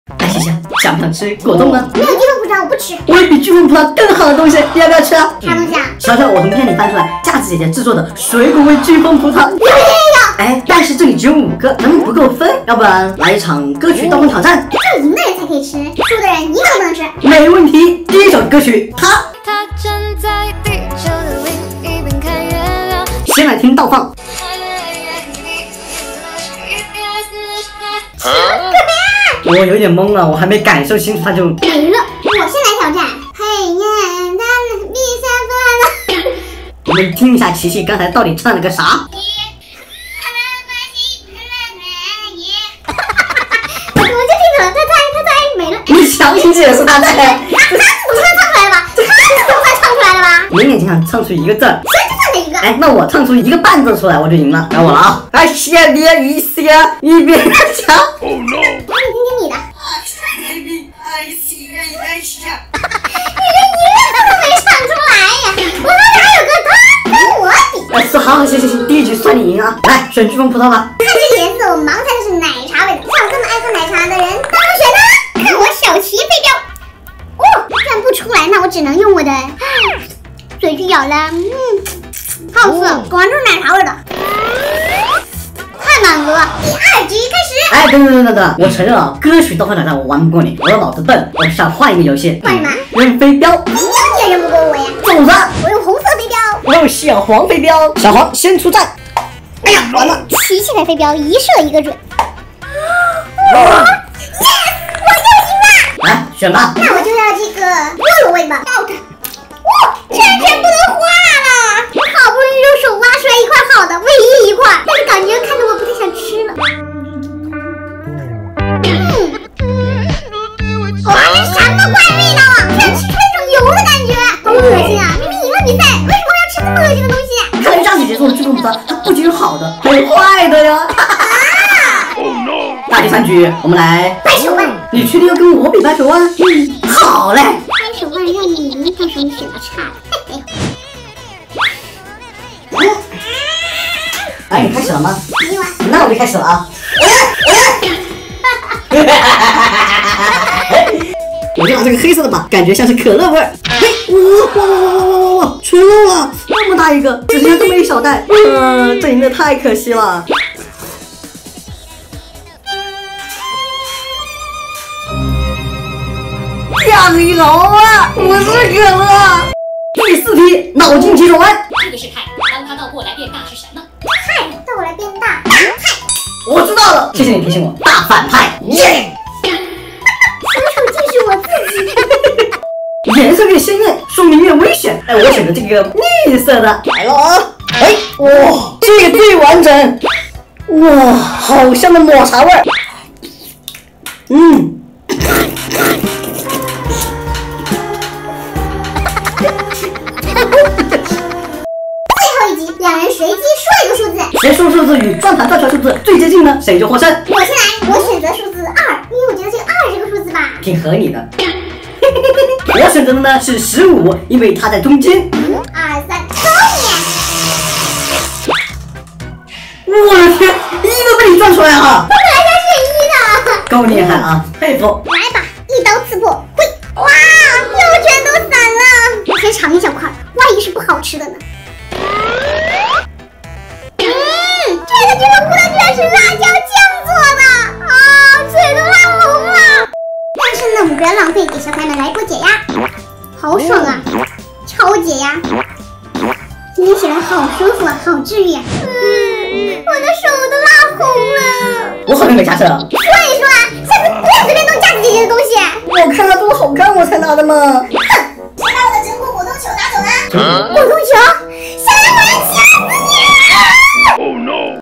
【架麼饞】巨型果凍大挑戰，甜美水果味VS奇葩醬油味，戳破瞬間太解壓了
小陽從冰箱裡翻出了架子姐姐做的巨型果凍，和奇奇進行歌曲倒放挑戰來分果凍，贏得可以選自己喜歡的，甜美水果味VS奇葩醬油味，吃的簡直太解壓了~

大家好，這裡是【架麼饞】
這裡每天會和大家分享奇特有趣的美食。